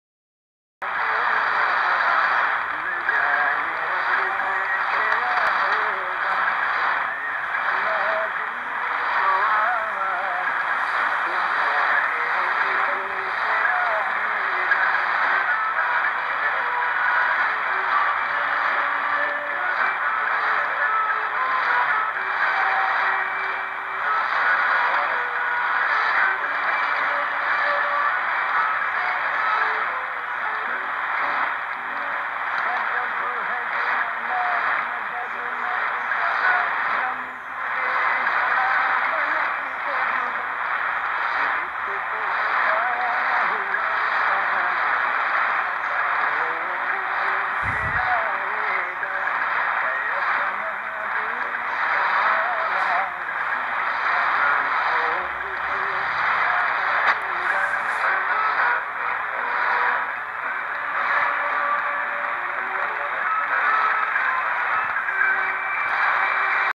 Bangladesh Betar - 13580 kHz
Receptor: Degen DE1103
Antena Parabólica
QTH: Londrina, Brasil
UTC 1845
Em 11 de agosto de 2014